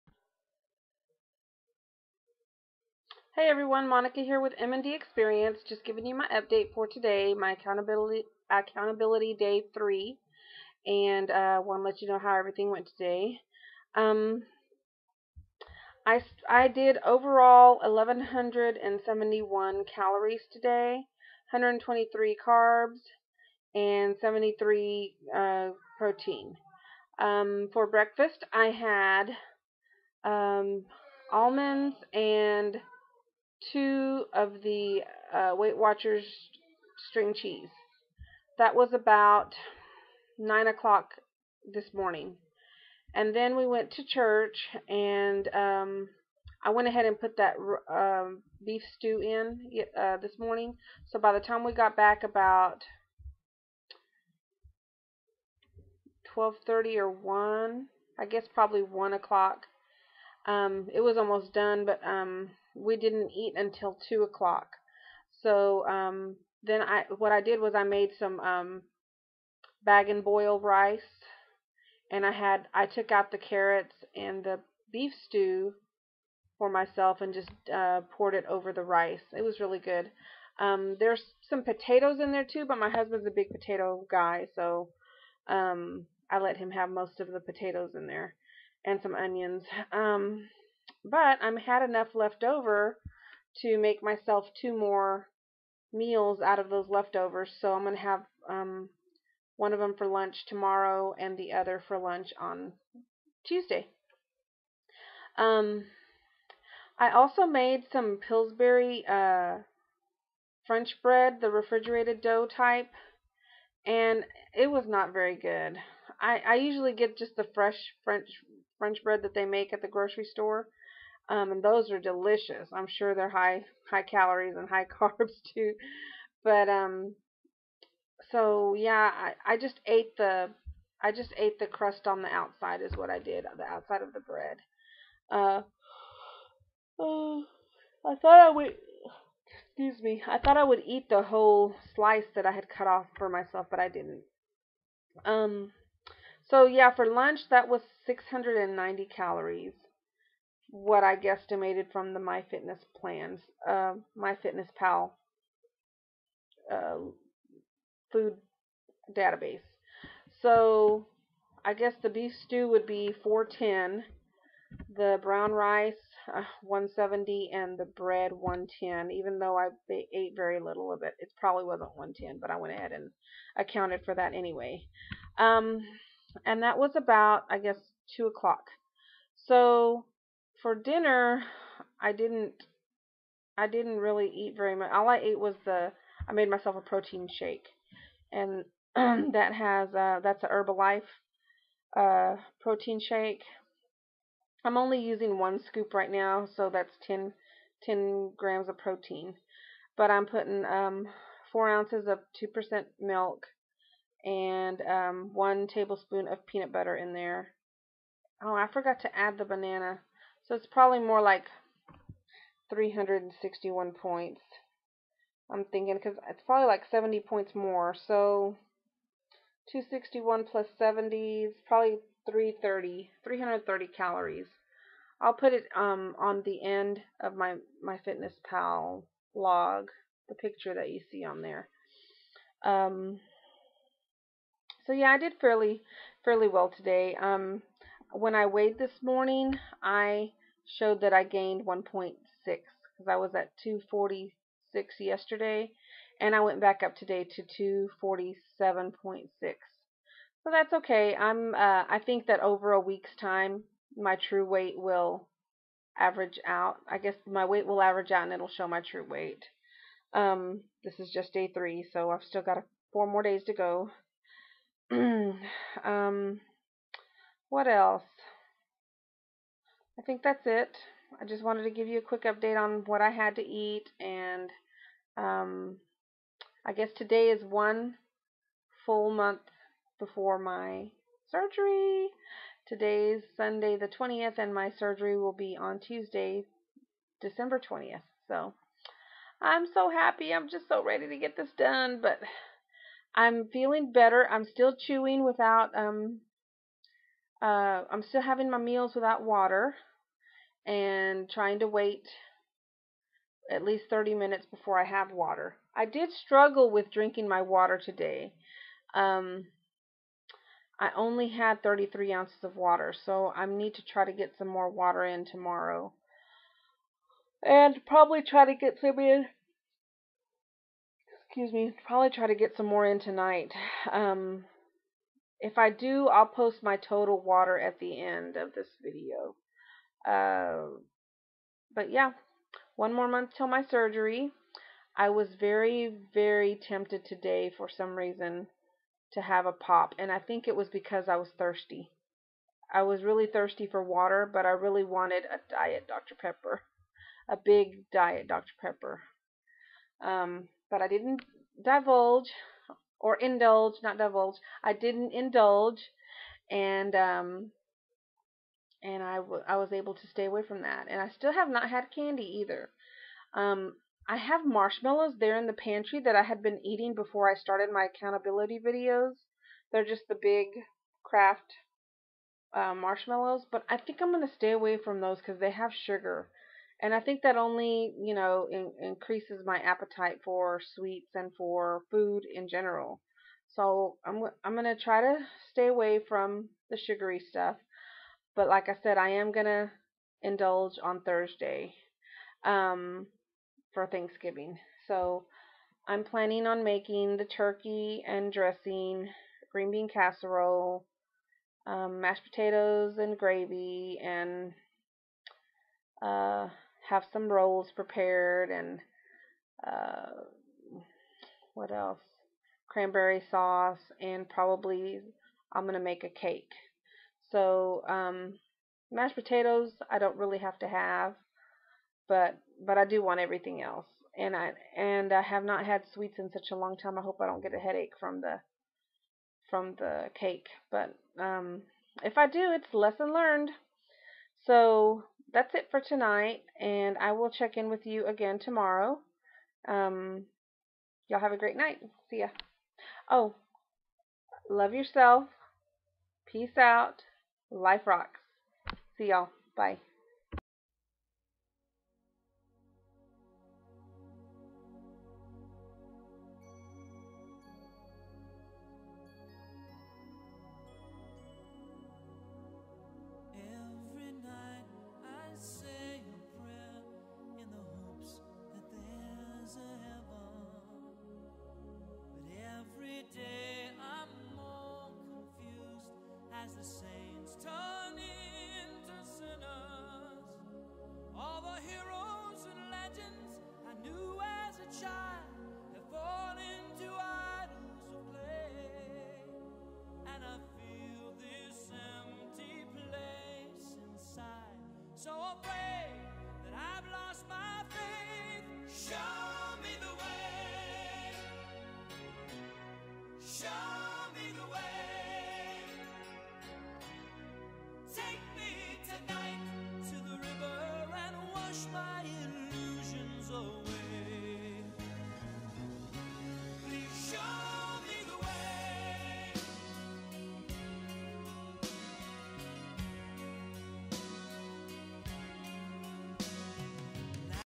Accountability Day 3: 30 days to Lap Band Surgery
Still logging my food. Struggled with wanting DDP because I was thirsty. Planning for Thanksgiving.